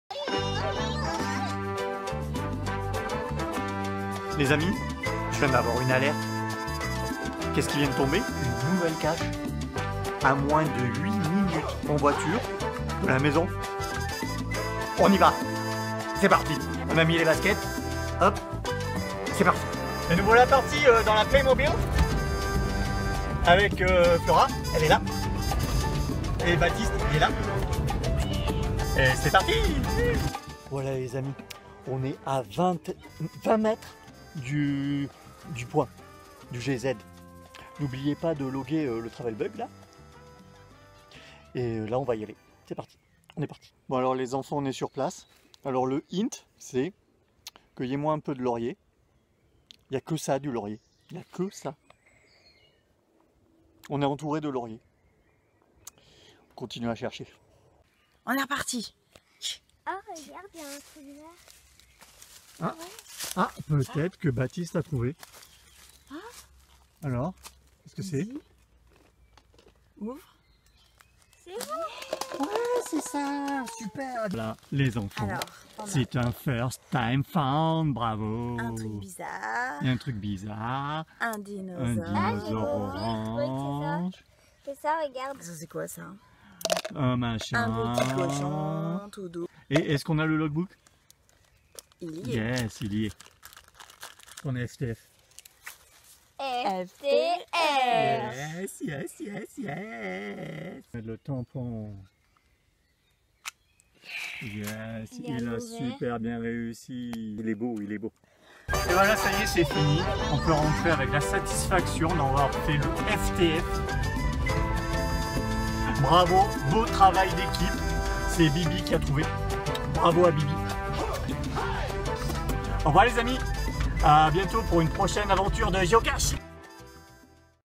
Font de Ribes MUZ
Un petit FTF (First Time Found) d'une géocache à la sauce (au laurier) KnifeMaster!!!